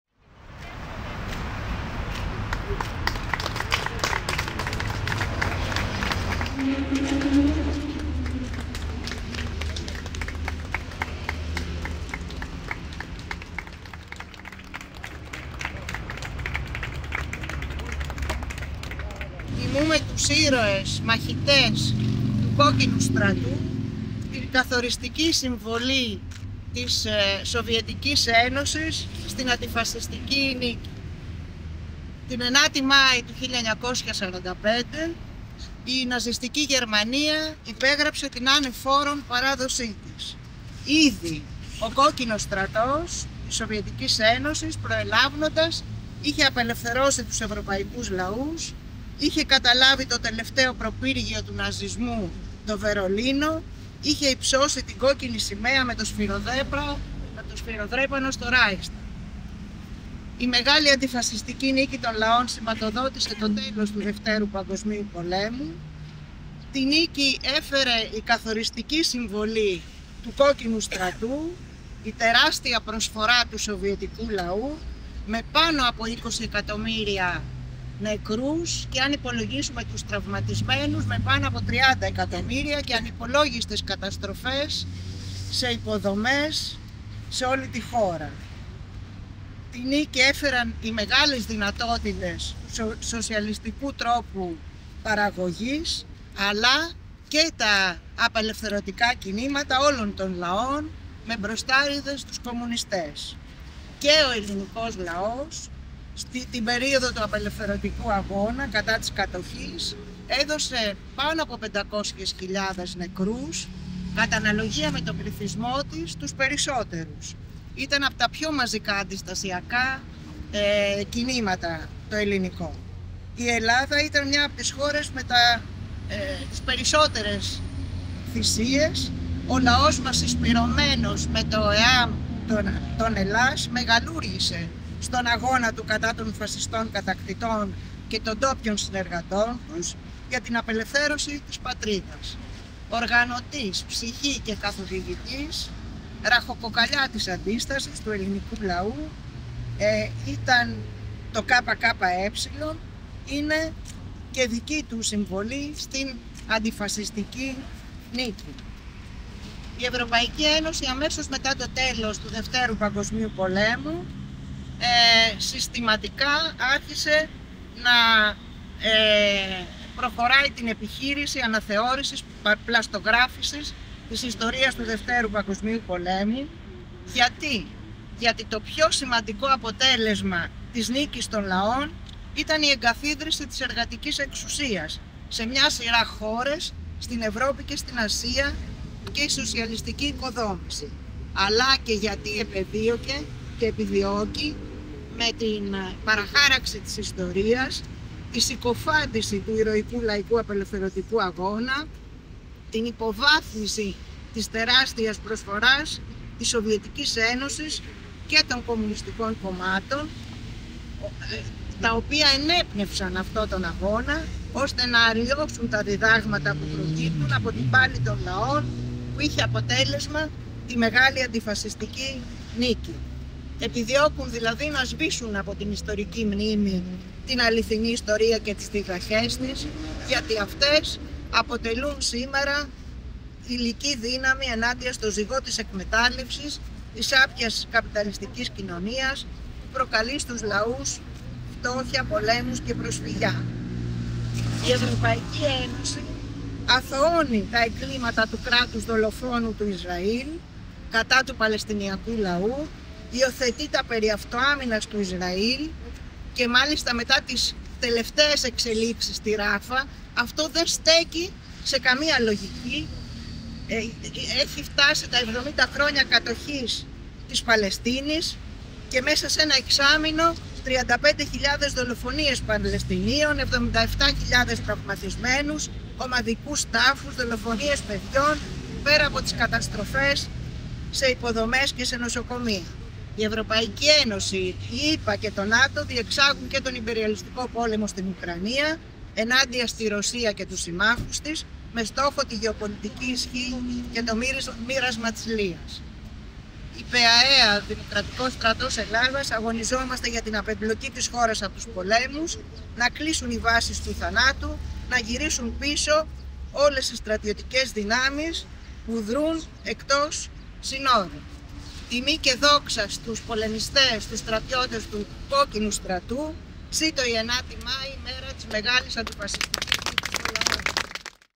Κατάθεση στεφάνων στο μνημείο του Σοβιετικού Στρατιώτη στην Καλλιθέα
Κατάθεση στεφάνων στο Μνημείο του Σοβιετικού Στρατιώτη στην Καλλιθέα, τιμώντας τη Μεγάλη Αντιφασιστική Νίκη των Λαών πραγματοποίησαν η ΤΕ Νότιου Τομέα της ΚΟ Αττικής του ΚΚΕ, το ΤΣ Νότιου Τομέα της ΚΝΕ όπως και η ΠΕΑΕΑ - ΔΣΕ.

Στεφάνι εκ μέρους του ΚΚΕ κατέθεσε ο Λευτέρης Νικολάου - Αλαβάνος, ευρωβουλευτής του Κόμματος και εκ νέου υποψήφιος.

Εκ μέρους της ΚΝΕ κατέθεσε στεφάνι η Δέσποινα Ρουμανέα, μέλος του ΚΣ και Γραμματέας του ΤΣ Νότιου Τομέα της ΚΝΕ και εκ μέρους της ΠΕΑΕΑ - ΔΣΕ η Ρούλα Ελευθεριάδου. Στεφάνι κατέθεσε επίσης και ο Σύλλογος «Εμείς που σπουδάσαμε στο Σοσιαλισμό».

Στην ομιλία της κατά πριν από την κατάθεση των στεφάνων, η πρόεδρος της ΠΕΑΕΑ-ΔΣΕ Τούλα Μητσέα, ανέφερε τα εξής:

«Τιμούμε τους ήρωες μαχητές του Κόκκινου Στρατού, τη καθοριστική συμβολή της Σοβιετικής Ένωσης στην Αντιφασιστική Νίκη.

Την 9η Μάη του 1945, η ναζιστική Γερμανία υπέγραψε την άνευ όρων παράδοσή της. Ήδη ο Κόκκινος Στρατός της Σοβιετικής Ένωσης προελαύνοντας είχε απελευθερώσει τους ευρωπαϊκούς λαούς, είχε καταλάβει και το τελευταίο προπύργιο του ναζισμού, το Βερολίνο, είχε υψώσει την κόκκινη σημαία με το σφυροδρέπανο στο Ράιχσταγκ.

Η Μεγάλη Αντιφασιστική Νίκη των Λαών σηματοδότησε το τέλος του Β’ Παγκόσμιου ιμπεριαλιστικού Πολέμου. Τη Νίκη έφερε η καθοριστική συμβολή του Κόκκινου Στρατού, η τεράστια προσφορά του σοβιετικού λαού, με πάνω από 20 εκατ. νεκρούς, οι μεγάλες δυνατότητες του σοσιαλιστικού τρόπου παραγωγής.

Τη Νίκη έφεραν οι μεγάλες δυνατότητες του σοσιαλιστικού τρόπου παραγωγής αλλά και τα απελευθερωτικά κινήματα όλων των λαών, με μπροστάρηδες τους κομμουνιστές.

Και ο ελληνικός λαός, την περίοδο του απελευθερωτικού αγώνα κατά της Κατοχής, έδωσε πάνω από 500.000 νεκρούς. H Ελλάδα κατ αναλογία με τον πληθυσμό της έδωσε από τους περισσότερους, είχε από τα πιο μαζικά αντιστασιακά κινήματα. Ο λαός μας συσπειρωμένος στο ΕΑΜ-ΕΛΑΣ μεγαλούργησε με τον αγώνα του κατά των φασιστών κατακτητών και των ντόπιων συνεργατών τους για την απελευθέρωση της πατρίδας. Οργανωτής, ψυχή και καθοδηγητής, ραχοκοκαλιά της Αντίστασης του ελληνικού λαού ήταν το ΚΚΕ. Είναι η δική του συμβολή στην Αντιφασιστική Νίκη.

Η ΕΕ συστηματικά προχωράει την επιχείρηση πλαστογράφησης της Ιστορίας του Β’ ΠΠ. Γιατί το πιο σημαντικό αποτέλεσμα της Νίκης των Λαών ήταν η εγκαθίδρυση της εργατικής εξουσίας σε σειρά χωρών της Ευρώπης και της Ασίας και η σοσιαλιστική οικοδόμηση. Αλλά και γιατί επεδίωκε και επιδιώκει με την παραχάραξη της Ιστορίας τη συκοφάντηση του ηρωικού λαϊκού απελευθερωτικού αγώνα, την υποβάθμιση της τεράστιας προσφοράς της ΕΣΣΔ και των κομμουνιστικών κομμάτων, ώστε να αλλοιώσουν τα διδάγματα που προκύπτουν από την πάλη των λαών, που είχε αποτέλεσμα τη Μεγάλη Αντιφασιστική Νίκη. Επιδιώκουν να σβήσουν από την ιστορική μνήμη των λαών την αληθινή Ιστορία και τις διδαχές της, γιατί αυτές μπορεί να γίνονται σήμερα υλική δύναμη του αγώνα ενάντια στον ζυγό της εκμετάλλευσης, της σάπιας καπιταλιστικής κοινωνίας που προκαλεί στους λαούς φτώχεια, πολέμους και προσφυγιά.

Η ΕΕ αθωώνει τα εγκλήματα του κράτους-δολοφόνου του Ισραήλ, κατά του Παλαιστινιακού λαού. Υιοθετεί τα περί "αυτοάμυνας" του Ισραήλ, που έχει επιβάλει στην Παλαιστίνη 70 χρόνια κατοχής, έχει φτάσει μέσα σε ένα εξάμηνο τις 35 χιλιάδες δολοφονίες Παλαιστίνιων και τις 77 χιλιάδες τραυματισμούς, τους ομαδικούς τάφους, δολοφονίες παιδιών, πέρα από τις καταστροφές σε υποδομές, ακόμη και σε νοσοκομεία.

ΕΕ, ΗΠΑ και ΝΑΤΟ διεξάγουν τον ιμπεριαλιστικό πόλεμο στην Ουκρανία ενάντια στη Ρωσία και στο υπό διαμόρφωση ευρασιατικό μπλοκ, με στόχο τη γεωπολιτική ισχύ και το μοίρασμα της λείας.

Η ΠΕΑΕΑ - ΔΣΕ αγωνίζεται για την απεμπλοκή της χώρας από τους πολέμους, να κλείσουν οι βάσεις του θανάτου, να γυρίσουν πίσω όλες οι στρατιωτικές δυνάμεις που δρουν εκτός συνόρων.

Τιμή και δόξα στους πολεμιστές, τους στρατιώτες του Κόκκινου Στρατού, ζήτω η 9η Μάη, μέρα της μεγάλη Αντιφασιστικής Νίκης!».